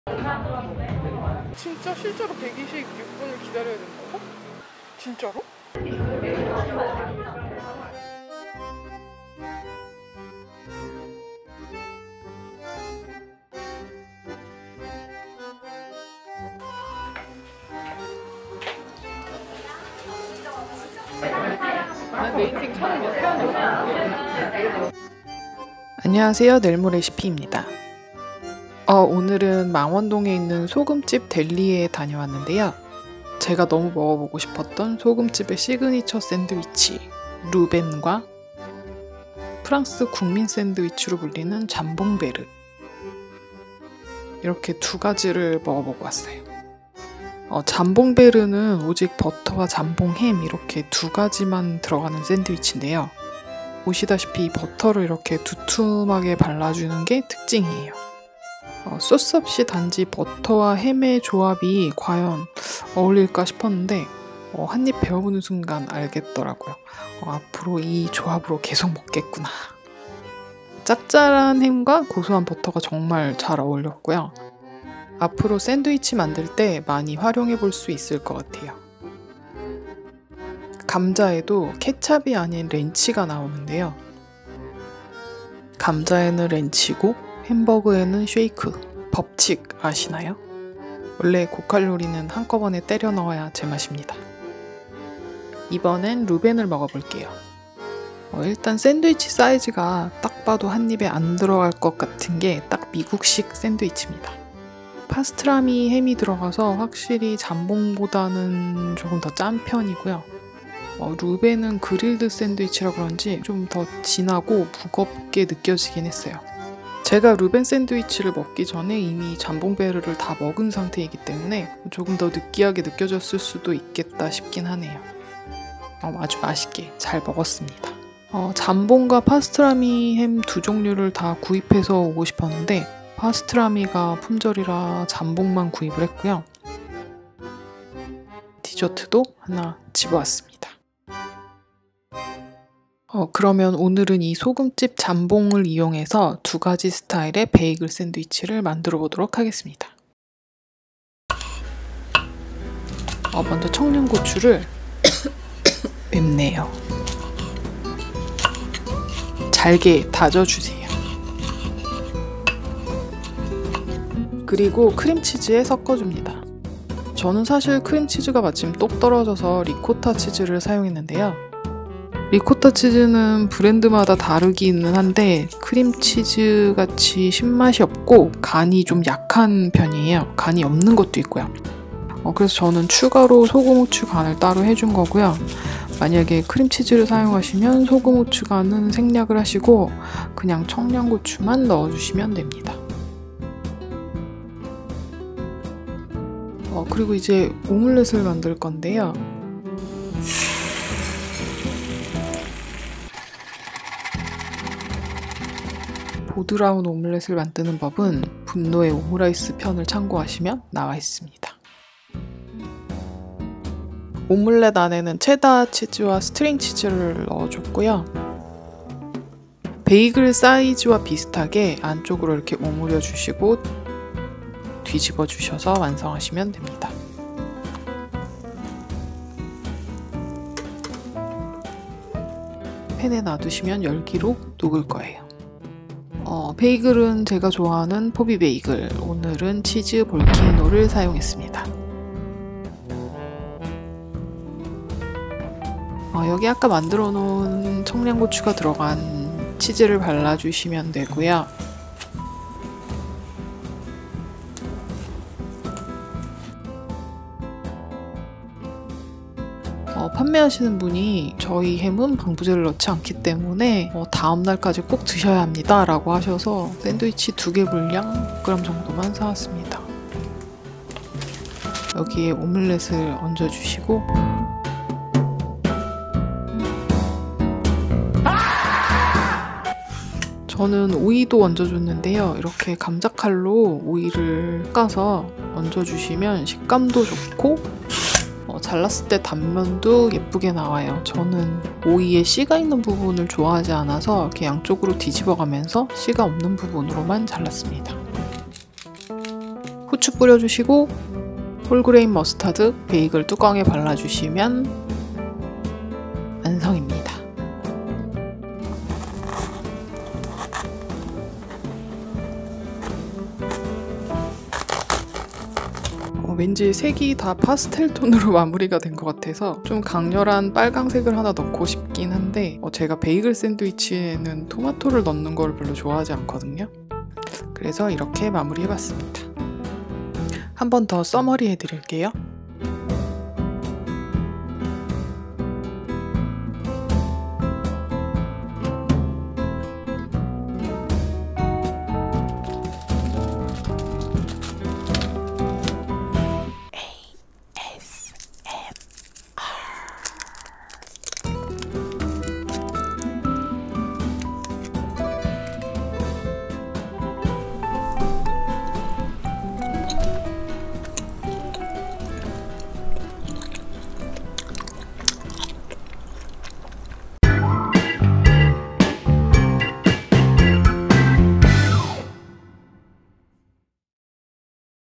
소금집 잠봉 베이글 크림치즈 샌드위치 만들기
안녕하세요 넬모레시피입니다.
소금집 잠봉으로 만들어 본 베이글 샌드위치에요.

[잠봉 베이글 샌드위치 재료]
베이글 1개, 크림치즈 3 스푼, 청양 고추 1개, 계란 2개, 슬라이스 체더치즈 한 장, 스트링치즈 한 개, 소금집 잠봉햄, 오이 한 개, 홀그레인 머스터드 1 티스푼

[잠봉 베이글샌드위치레시피]
1. 베이글은 살짝 토스트 해둡니다
2. 크림치즈에 청양 고추를 다져서 넣어주세요
3. 계란 두 개를 풀어 체에 거르고 오믈렛을 만들어주세요.
4. 오믈렛에 체다치즈와 스트링치즈를 넣어서 잘 오므려주세요
5. 베이글에 크림치즈를 바르고
6. 소금집 잠봉햄을 올립니다 (50g 다섯 장 정도)
7. 치즈가 녹은 오믈렛을 올리고
8. 감자칼로 슬라이스 한 오이를 올려주고
9. 오이 위에 후추 찹찹
10. 베이글 뚜껑에 홀그레인 머스터드를 바르고 덮어주면 완성

🤗저는 크림치즈가 떨어져서 리코타에 소금 후추 간을 해서 사용했어요.
(크림치즈의 진한 맛이 더 어울리는 샌드위치입니다)

🤗재료는 집에 있는 걸로 바꿔쓰셔도 무방합니다.
잠봉햄 --- 일반 햄🥓
청양 고추 --- 할라피뇨🫑 (사실 할라피뇨 구하기가 더 어렵죠 ^^)
슬라이스 치즈🧀 --- 집에 있는 거 아무거나 넣으세요
스트링 치즈 --- 모짜렐라 있으시면 더 좋아요

냉장고에 있는 재료 활용해보세요!

🙋🏻‍♀️🙋🏻‍♀️🙋🏻‍♀️ 블로그, 타 SNS에 사용 시 꼭 출처를 밝혀주세요